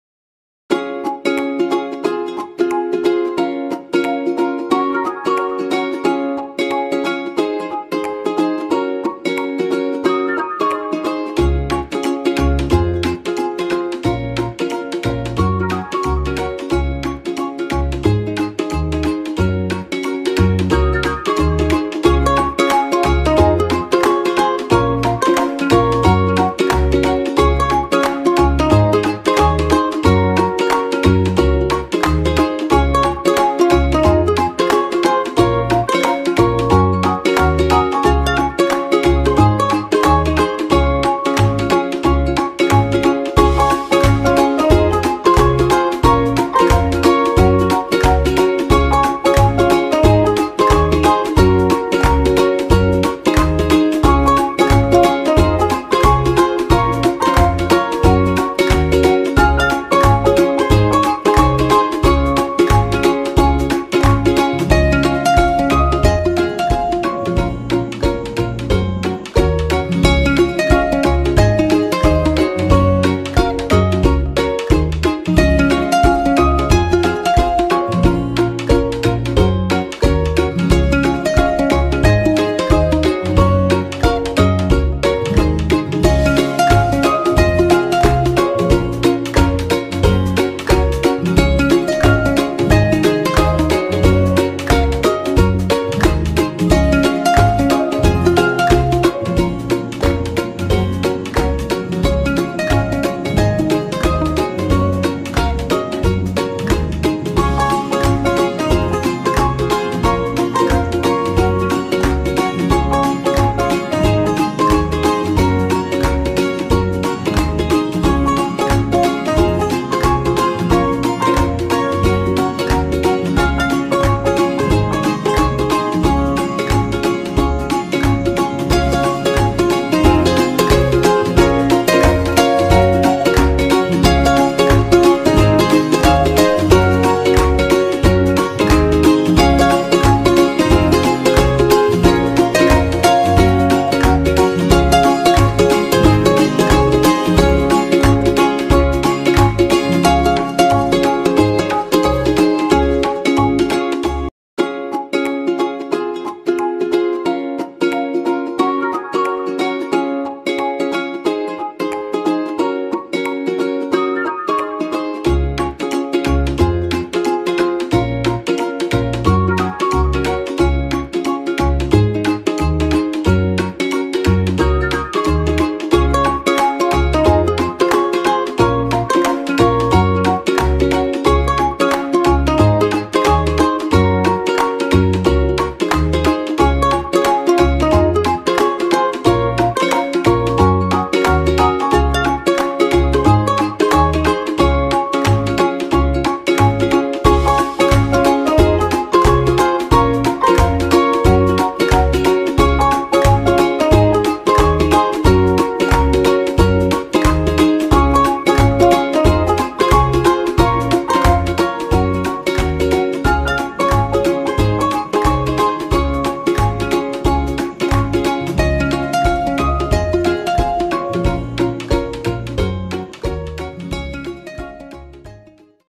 Quarantine ulam ideas (Tuna rebisco nuggets & Tuna repolyo recipes)
Quarantine ulam ideas (Tuna rebisco nuggets & Tuna repolyo recipes) puro nalng de lata mga besh?